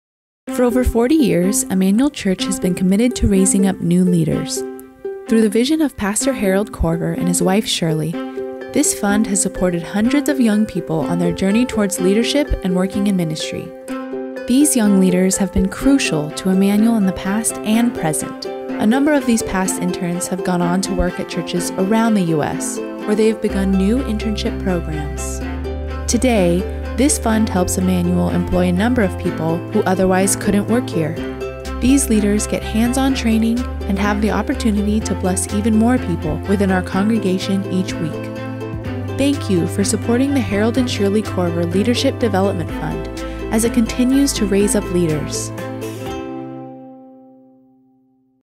The Harold & Shirley Korver Leadership Development Fund
Learn more about leadership development at Emmanuel Church, and how this fund has helped provide for decades!